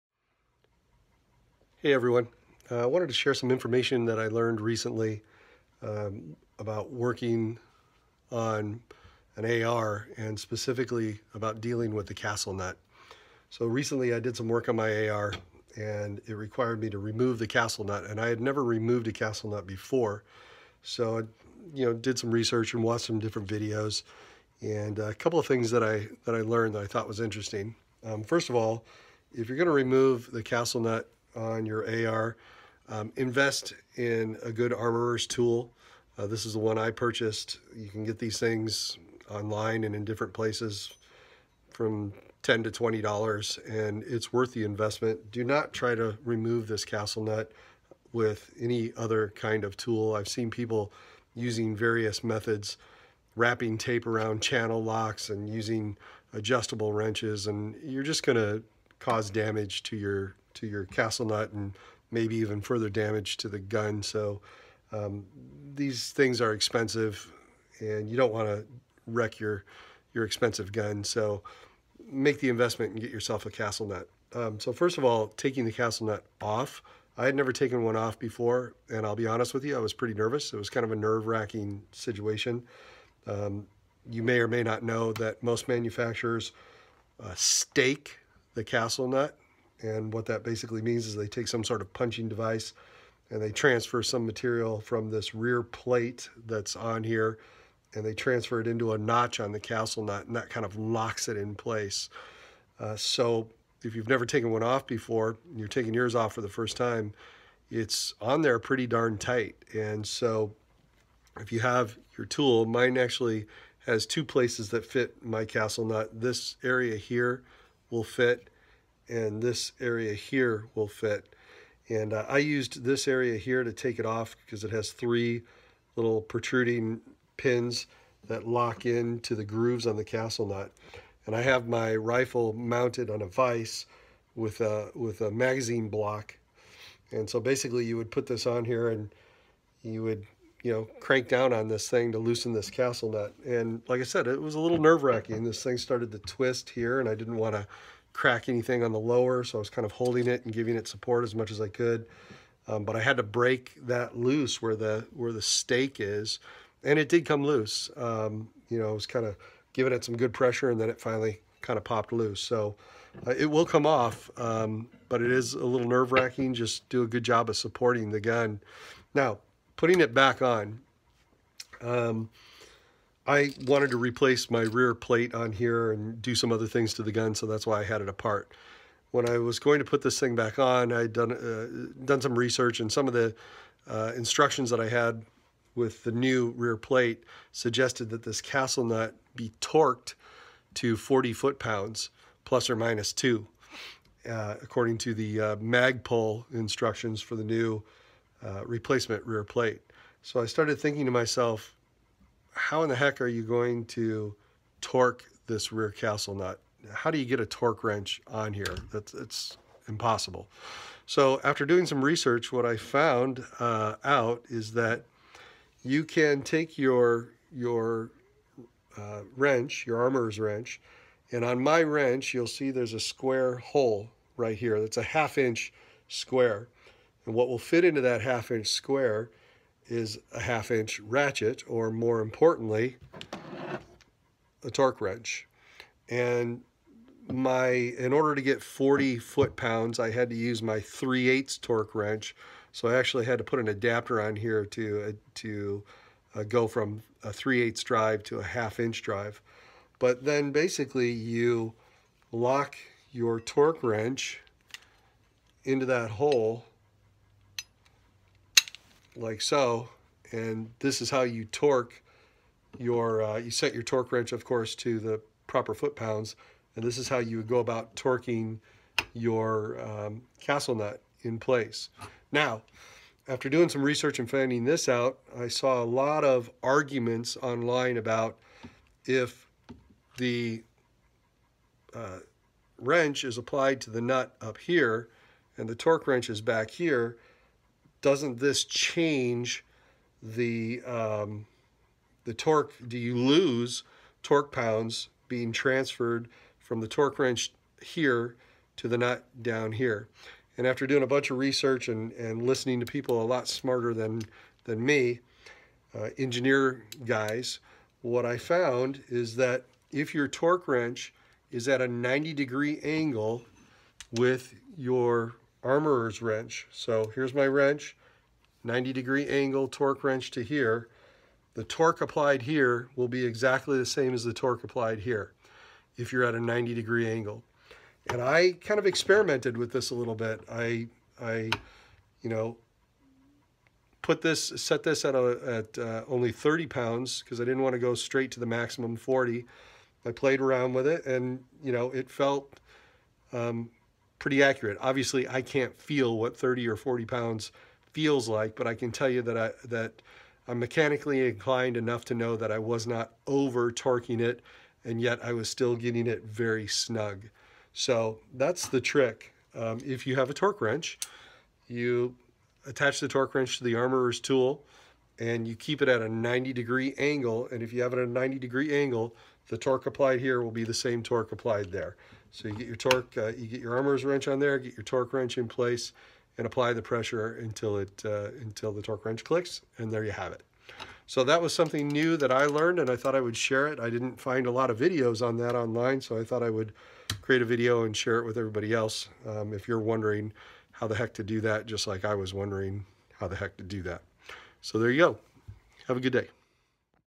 AR15 Castle Nut
Loosening an AR15 castle nut and torquing the nut after it is reinstalled.